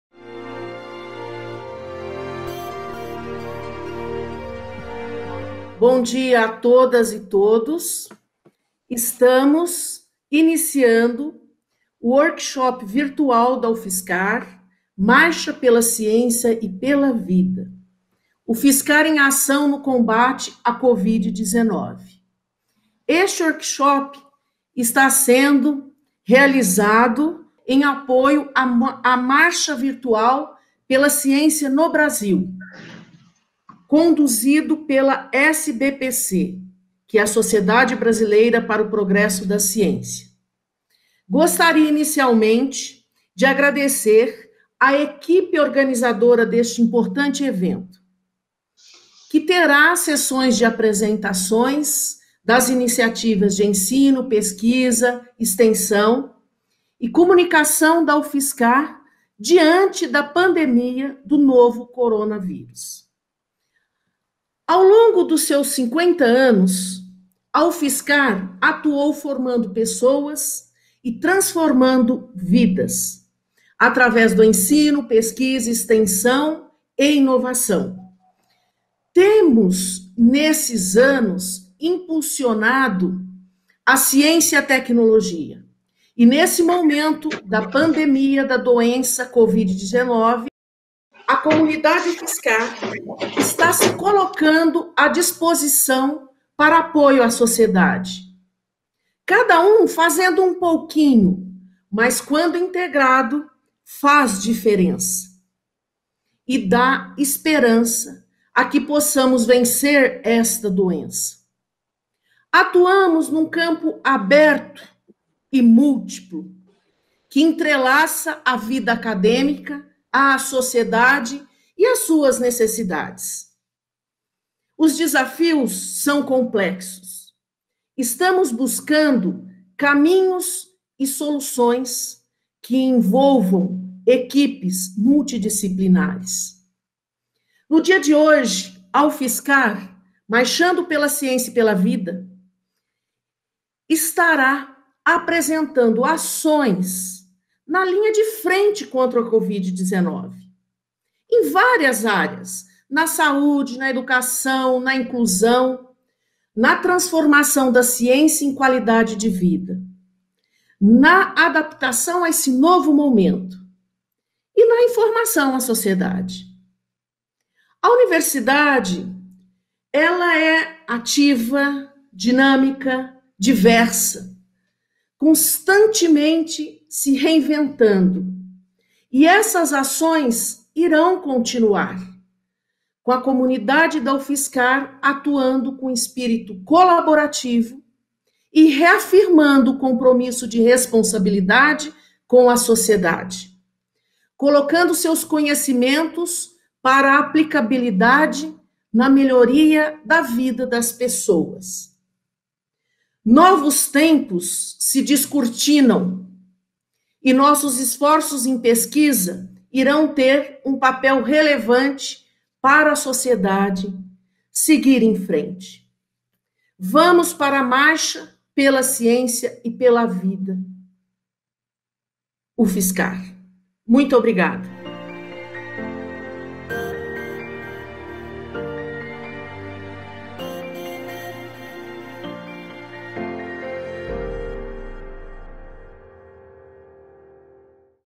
Abertura - Marcha pela Ciência e pela Vida: UFSCar em ação no combate à COVID-19
Abertura: Reitora da UFSCar, Wanda Hoffmann

Workshop virtual - Marcha pela Ciência e pela Vida: UFSCar em ação no combate à COVID-19

- No Instagram: @reitoria.ufscar